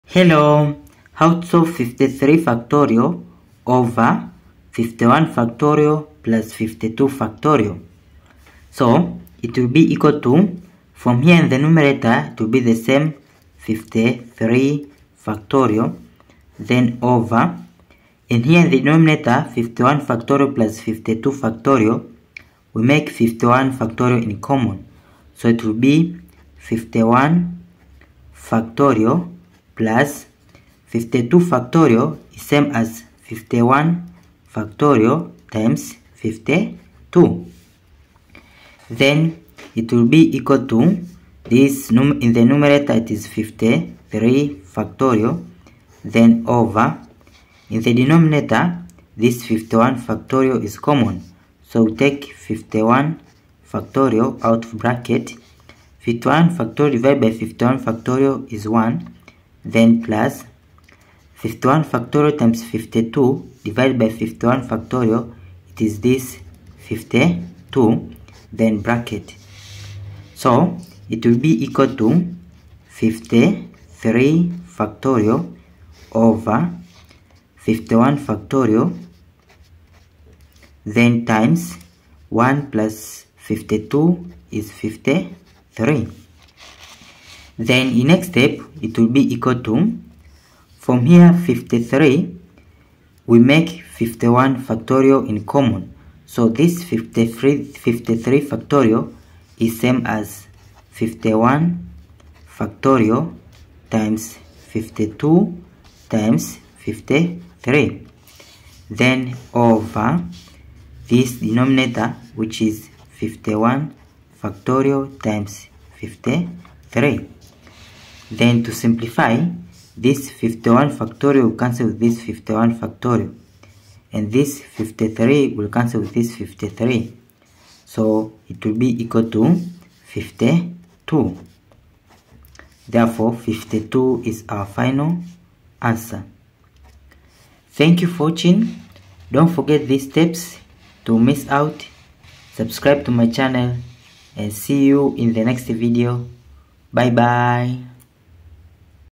A Nice Factorial Problem
How solve 53!/(51! + 52!) in this math Problem